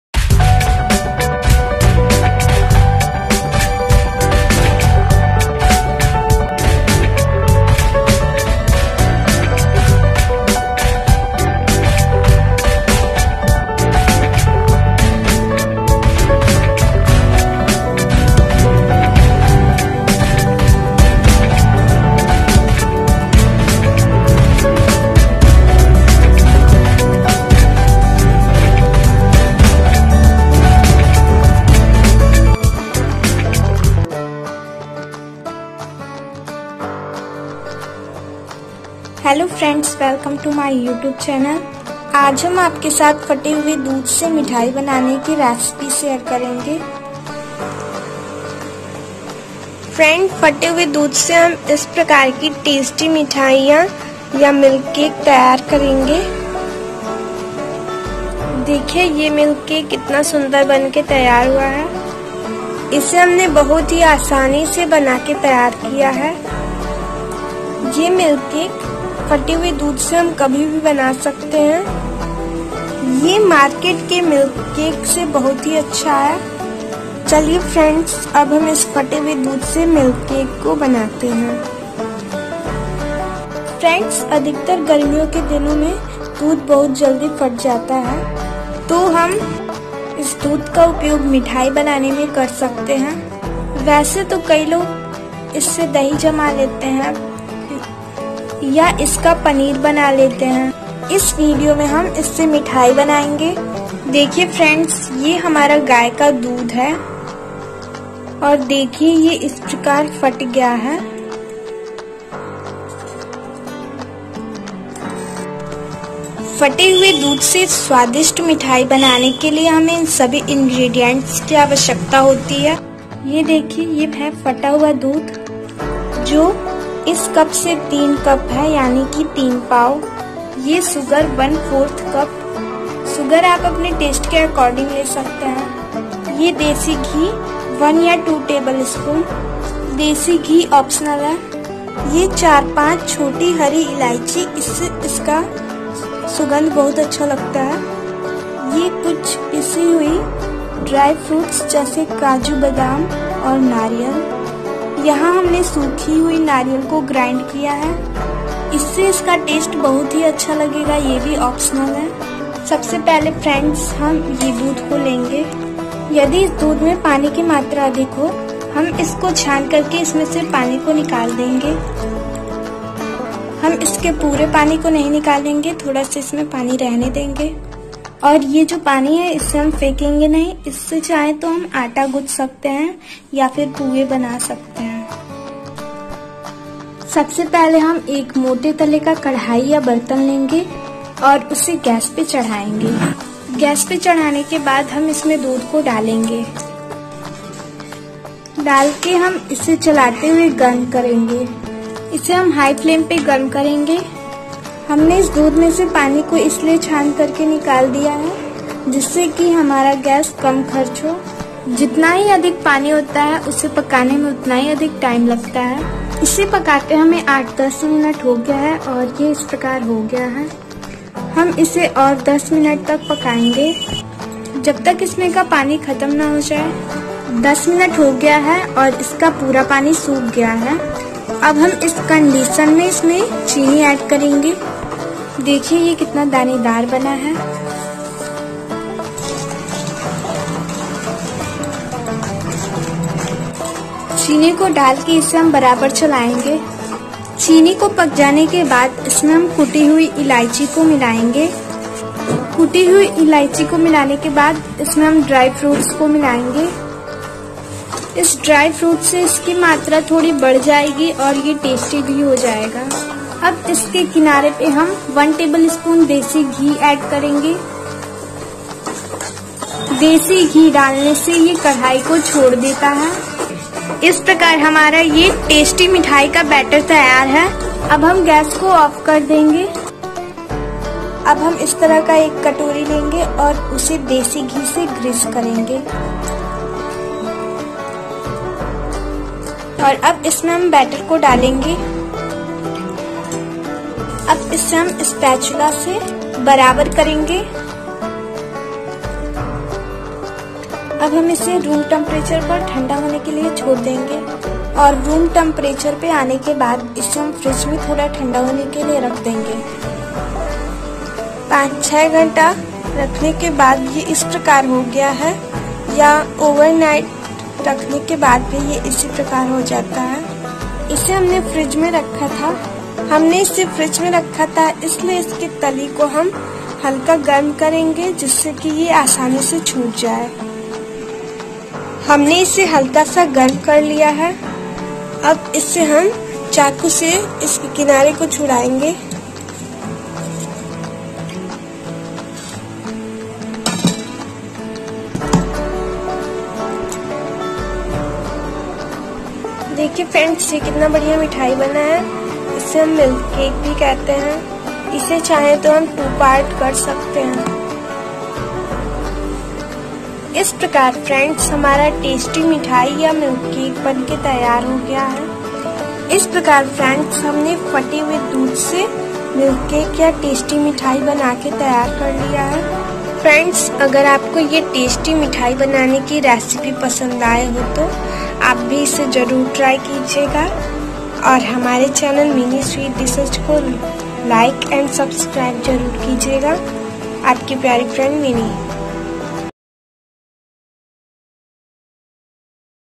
फटे हुए दूध से बनाए स्वादिष्ट मिठाई(मिल्क केक) |Milk cake recipe| How to make tasty sweet
फटे हुए दूध का हम चाहे तो कई तरह से उपयोग कर सकते हैं।
जैसे दही बनाने में या मिठाईयां बनाने मैं|इस वीडियो में हम इस फटे हुए दूध से स्वादिष्ट मिठाई(मिल्क केक) बनाने की रेसिपी शेयर करेंगे।इस फटे हुए दूध से हम चाहे तो छेना का मिठाई बना सकते हैं।यह जो मिल्क केक बना रहे हैं हम वह बहुत ही टेस्टी है। मार्केट के मिल्क केक से भी ज्यादा टेस्टी है।

1). Delicious desserts with cracked milk.
2). Tasty sweet (milk cake) recipe
3). How to make milk cake at home with cracked milk
4). Delicious sweet recipe